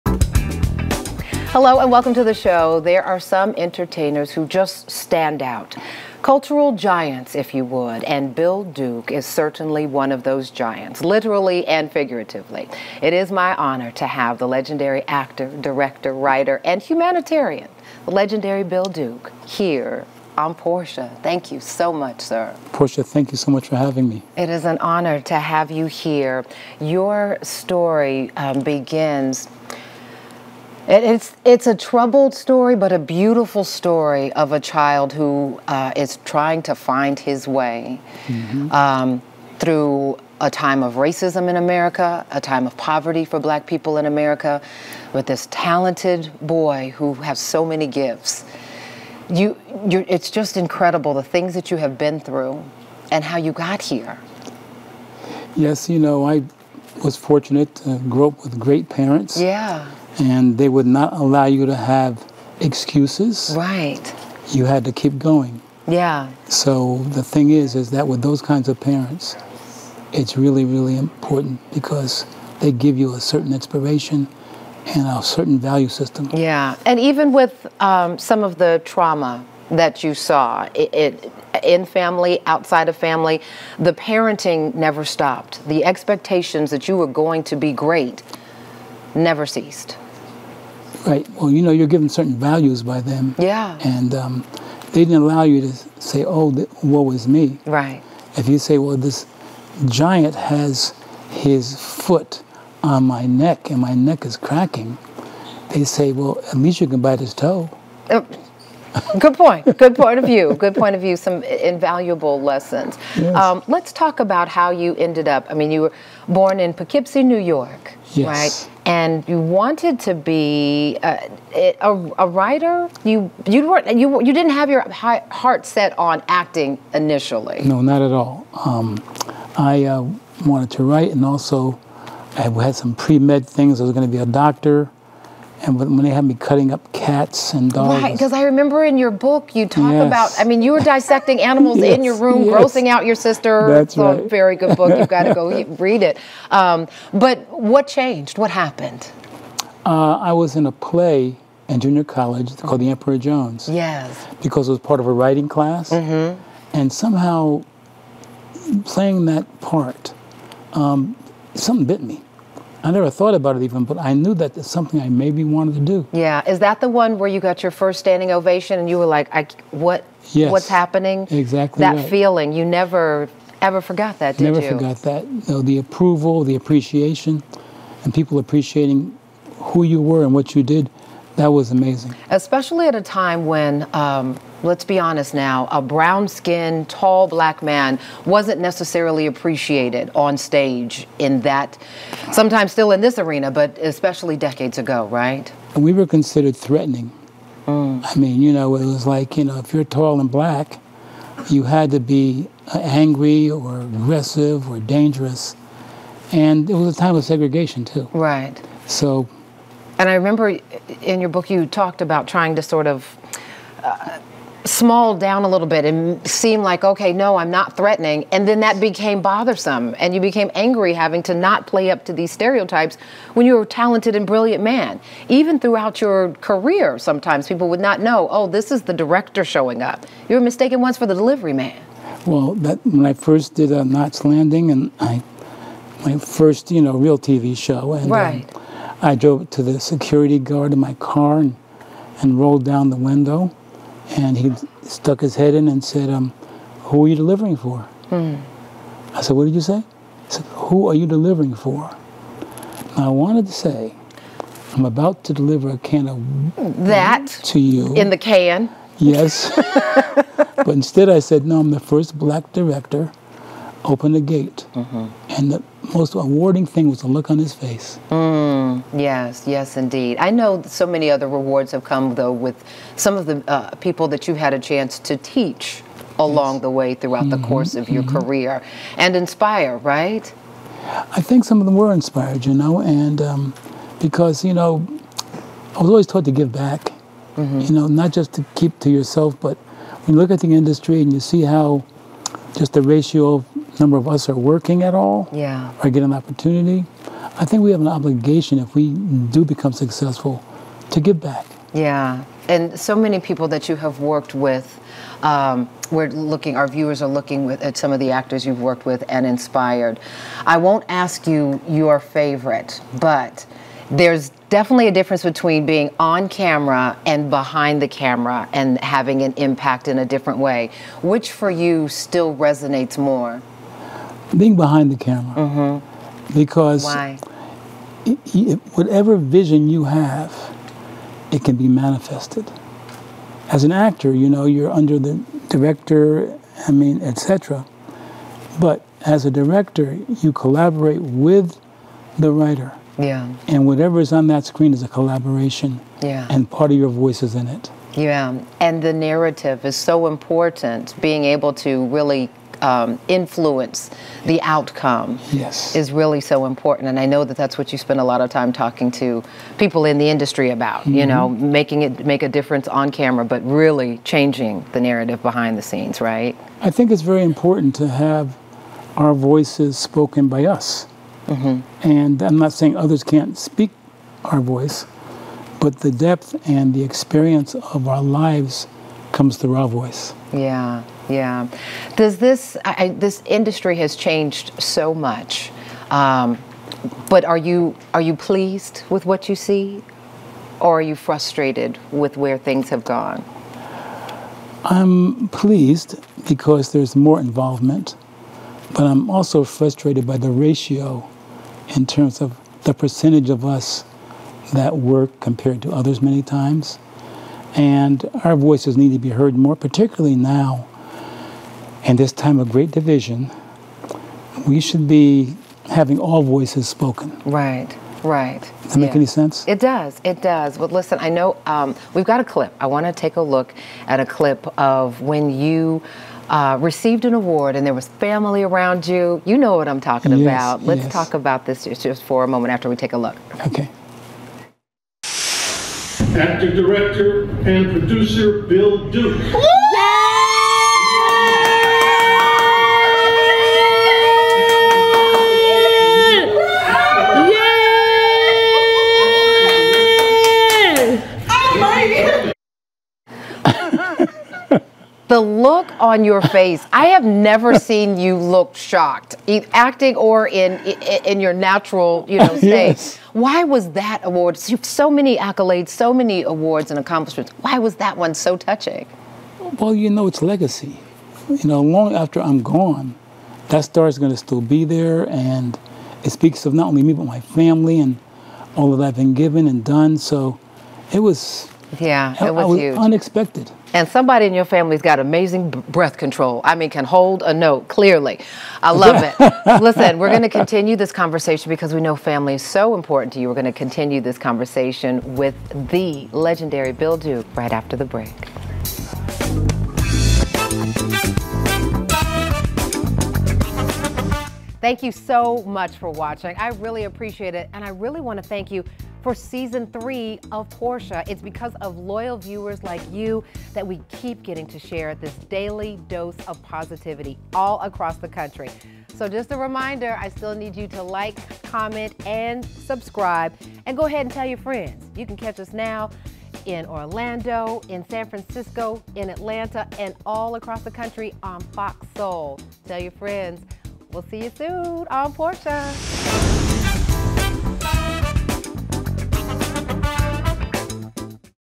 Legendary Actor Bill Duke Recalls Being Stereotyped While Working In Hollywood
Legendary actor, director, writer, and humanitarian Bill Duke sits down for an intimate conversation with Portia. The iconic entertainer gets real about the life lessons he’s learned from family, the training he’s giving to other actors, and how he’s educating the public about syphilis.

Featuring @OfficialBillDuke